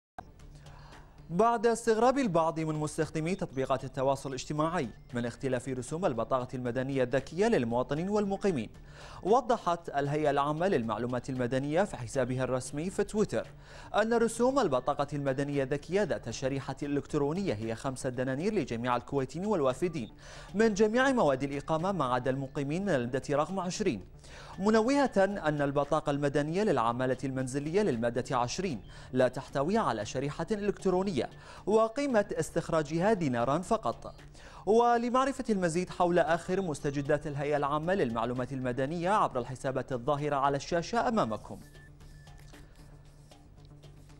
البطاقة المدنية للمواطنين والوافدين بـ 5 دنانير , وللمادة 20 بـ دينارين
تلفزيون،الكويت: البطاقة المدنية للمواطنين والوافدين بـ 5 دنانير , وللمادة 20 بـ دينارين